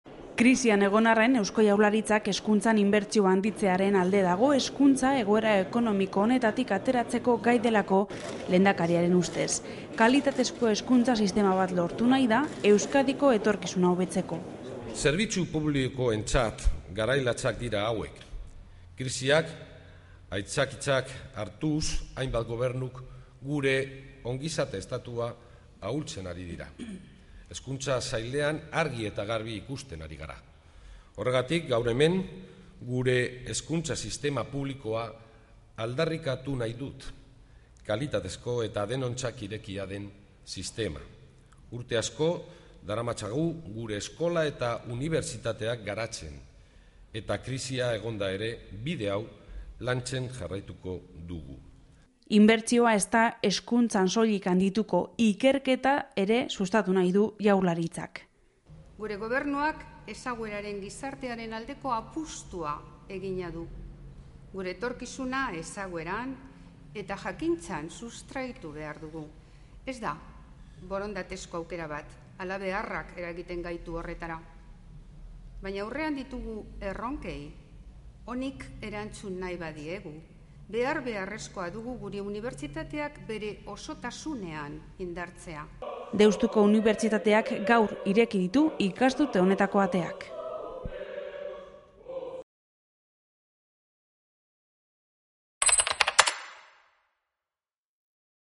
Hezkuntzako inbertsioa handitzearen alde agertu da lehendakaria
Lehendakariak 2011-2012 ikasturtea inauguratu du gaur Deustuko Unibertsitatean (Bilbo)

Patxi Lopezen ustez «inoiz baino garrantzitsuagoa da zerbitzu pubikoak aldarrikatzea, batez ere, kalitatezko hezkuntza, gizarte kultuagoa, askeagoa eta lehiakorragoa eraikitzeko oinarri gisa.Azaldu duenez, egoera ekonomikoa txarra izan arren «hezkuntzari zuzendutako inbertsioak mantentzea eta haztea, eta  sistema ere modernizatzea lortzen ari gara, eta hala izaten jarraituko du etorkizunean».